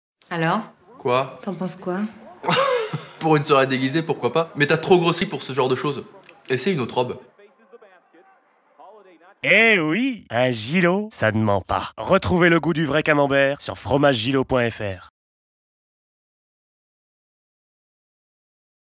Publicité TV : Spot essayage robe "un Gillot, ça ne ment pas" Camembert AOP
Publicité réalisée pour la fromagerie Gillot dans le cadre d'une campagne TV nationale.

Nb de Films : 3
Durée : 0:19 min
Nb de jours de tournage : 2 jours
Nb de jours de post-production : 3 jours
DA / Réalisation : F.Catteau
Création graphique : B.Guillaume
Montage vidéo : F.Catteau
Design sonore : D.Taburet
Animation / Compositing / Mixage son : B.Guillaume / D.Taburet
Production : 2M Productions

Extraits sur YouTube de "La Minute Vraie"